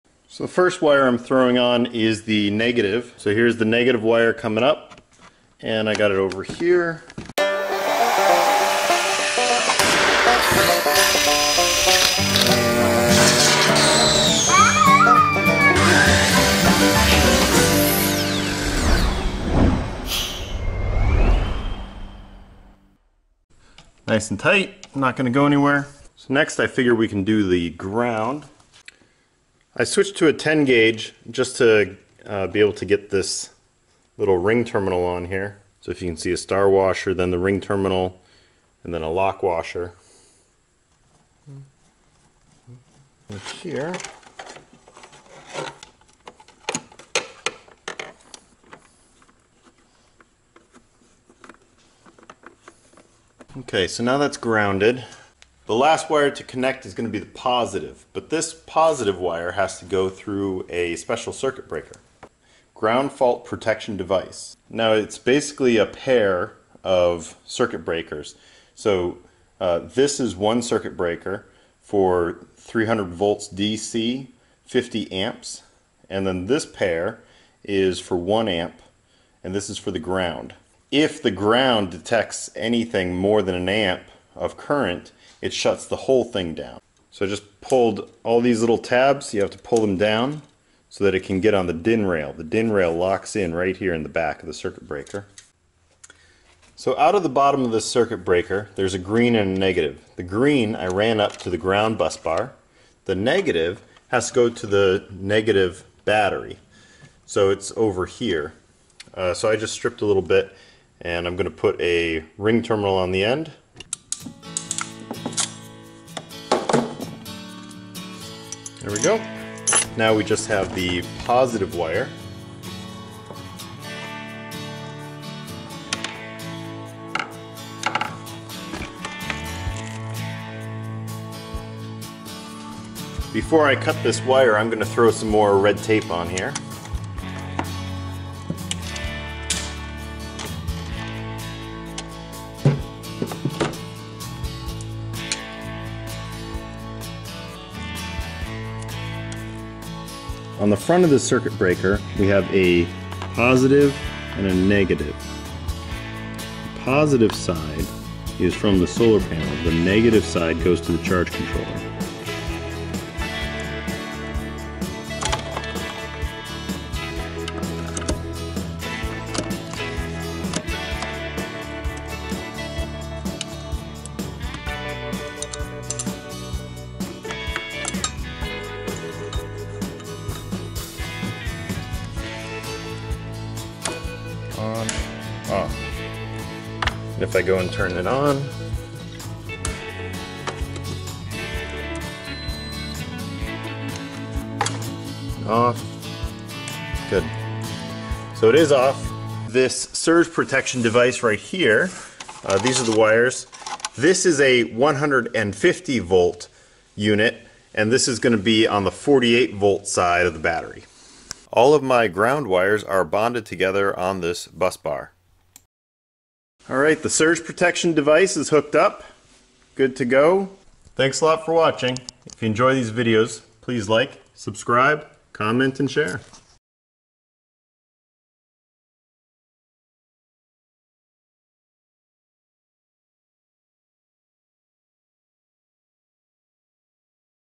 Wiring the Solar with a Couple of Safety Devices
Now that the solar wires are run into my DIY powerwall, I need to connect them inside my DC load center.  This load center is like a central hub for the DC wires, and a circuit breaker box.  I'm able to connect up my Victron charge controller 250/100, SPD (surge protection device), and my GFPD (ground fault protection device).  Note, in the video I mention the SPD being 150v, it is not, it's 115v, sorry about that.

The DC load center is made by Midnite Solar.  I have a video on installing it into my DIY powerwall

The charge controller I'm using is a Victron MPPT 250/100.  It has maximum power point tracking, works with lithium batteries, takes up to 250 volts from the solar PV, and can send up to 100 amps to the batteries.  It is quite an impressive unit.  I have two videos about the charge controller

SPD (Surge Protection Device)

GPPD (Ground Fault Protection Device)

DC Load Center

Ring Terminals

Crimp Tool

Victron Charge Controllers on Amazon